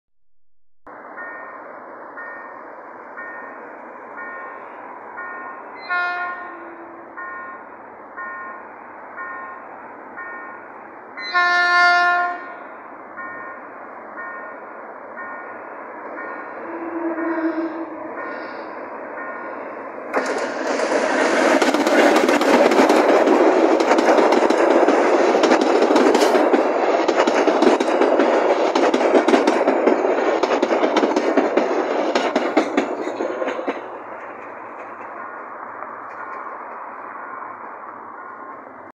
6524 - FERROVÍAS PASANDO POR PANAMERICANA Y ACCESO PILAR
Retiro - Villa Rosa

Vemos trabajando a la locomotora G22 E721 pasando por Panamericana y acceso Pilar a las 14:23 rumbo a Retiro